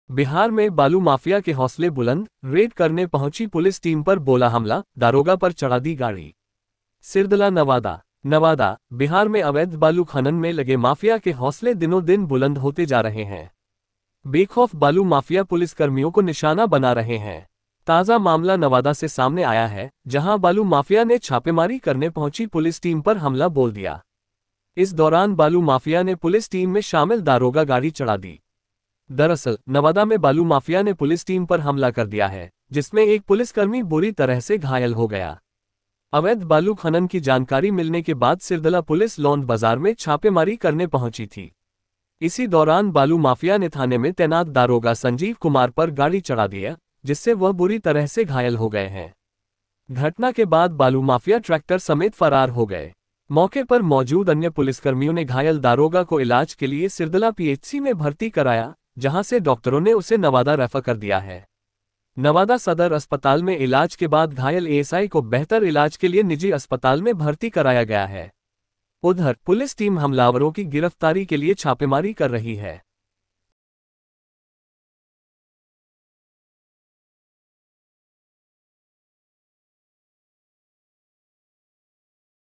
रेड करने पहुंची पुलिस टीम पर बोला हमला, दारोगा पर चढ़ा दी गाड़ी।सिरदला नवादा
बिहार में बालू माफिया के हौसले बुलंद: रेड करने पहुंची पुलिस टीम पर बोला हमला, दारोगा पर चढ़ा दी गाड़ी।
सिरदला नवादा
नवादा: बिहार में अवैध बालू खनन में लगे माफिया के हौसले दिनों दिन बुलंद होते जा रहे हैं। बेखौफ बालू माफिया पुलिसकर्मियों को निशाना बना रहे हैं। ताजा मामला नवादा से सामने आया है, जहां बालू माफिया ने छापेमारी करने पहुंची पुलिस टीम पर हमला बोल दिया।